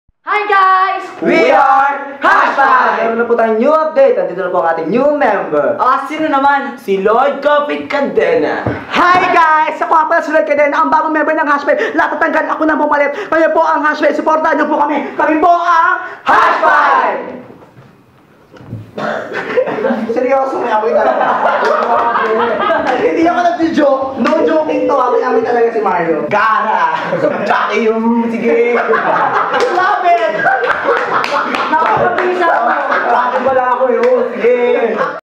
ANG BAGONG MEMBER NG HASHT5
May bagong recruit ang HASHT5, ipinakikilala ang kanilang bagong member, Ang Rapper Kween ng Paranaque, si Lloyd Cafe Cadena.

Abangan ang ITANONG MO with HASHT5, featuring your questions, ngayong October 14, 2015 sa aking YouTube Channel. pasabog ito!

HASHT 5 MEMBERS:
Marlou Arizala, Erick Oxford Ebreo, Vincent V. Binocas, Jhimwel Macanlalay, Carl Cee Jhay Eliero (ABSENT)

Special Thanks to Vladimir Dizon (Facebook Admin), Matt Nicoli and Lye Ocampo of Chicken Pork Adobo (ABS-CBN).

Grab a copy of my books "Eng Serep Megwele" and "Hopia" at your nearest bookstores nationwide or order online.